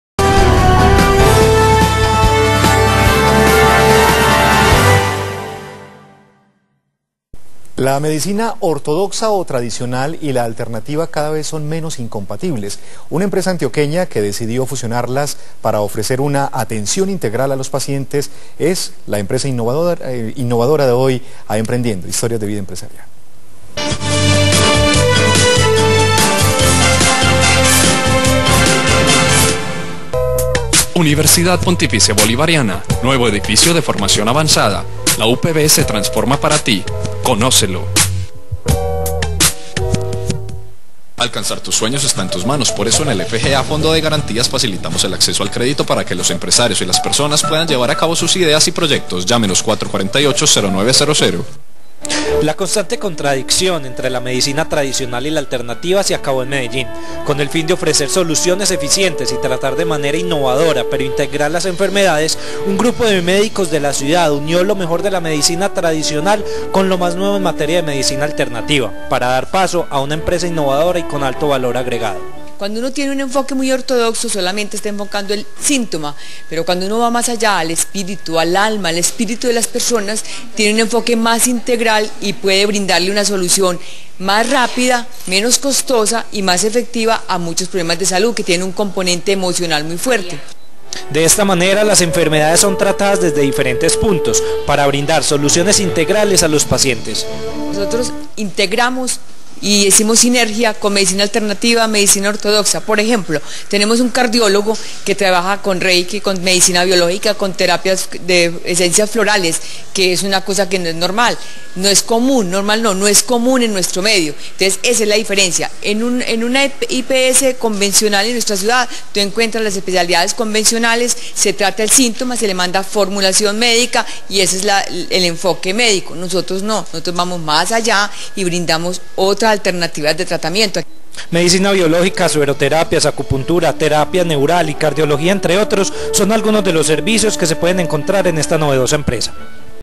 Medicina tradicional y medicina alternativa se unen por un bien común
Medicina tradicional y medicina alternativa se unen por un bien común. Hora13noticias